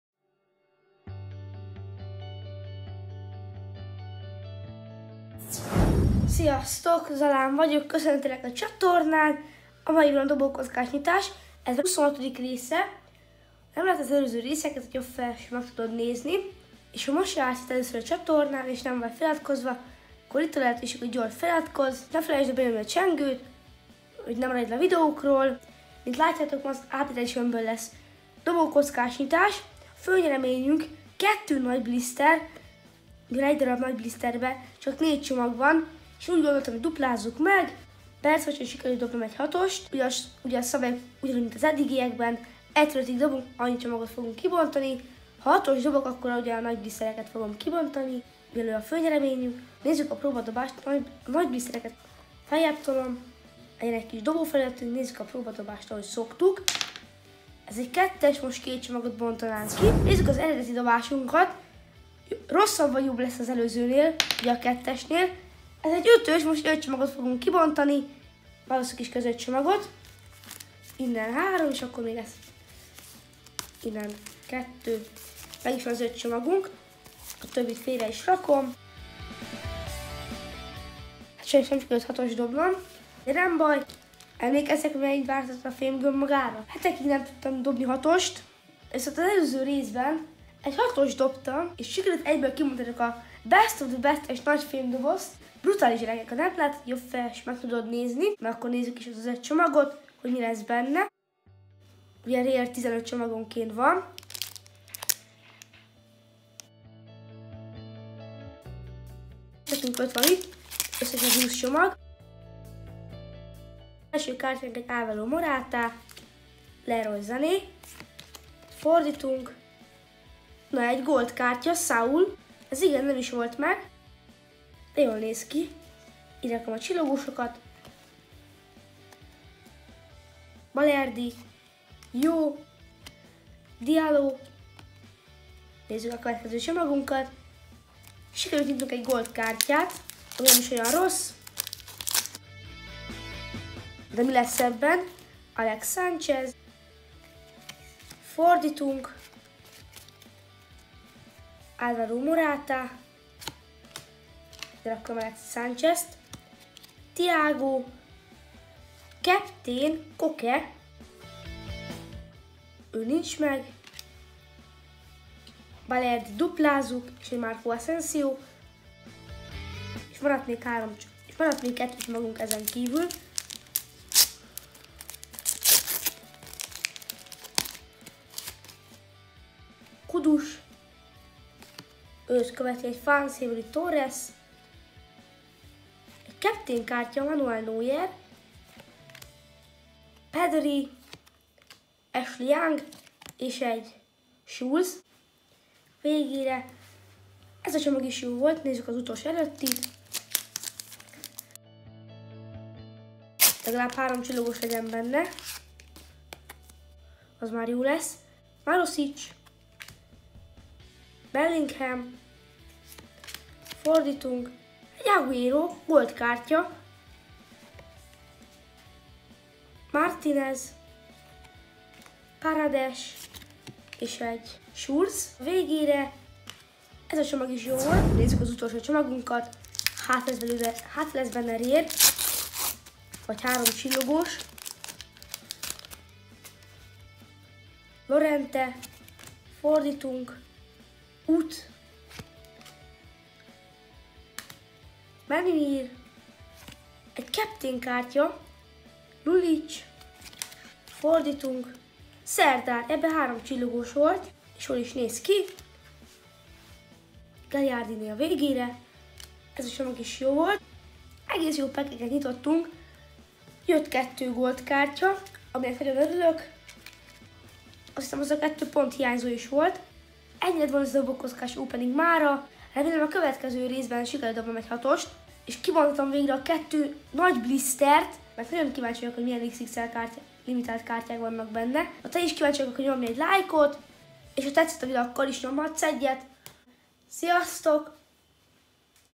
Focis Kártya Nyitás DOBÓKOCKÁVAL 26# ??!| Arenalyn XL 2021| Panini Fifa 365 UPDATE EDITION csomagok
Panini 2021Fifa 365 Adrenalyn xl focis kártyák ki bontás|
Ha írni szeretnél vagy cserélni akkor
itt tudsz nekem írni
vagy
Facebook Oldalam kövess be bátran :) és oszd meg a videót ha tetszett